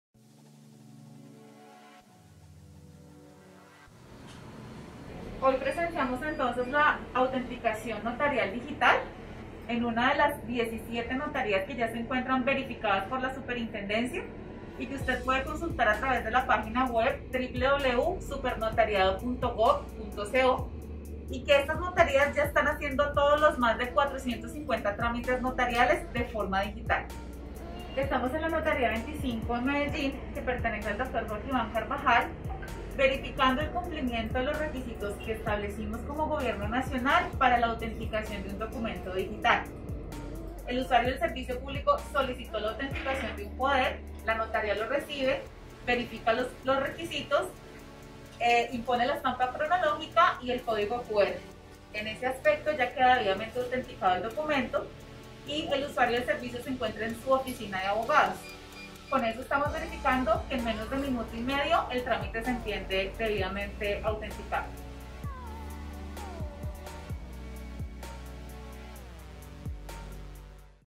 ¿Cómo es el proceso de autenticación digital?
La Superintendente de Notariado y Registro, Goethny Fernanda García, explica cómo es el proceso de autenticación digital, uno de los más de 450 trámites que se podrán realizar de manera virtual.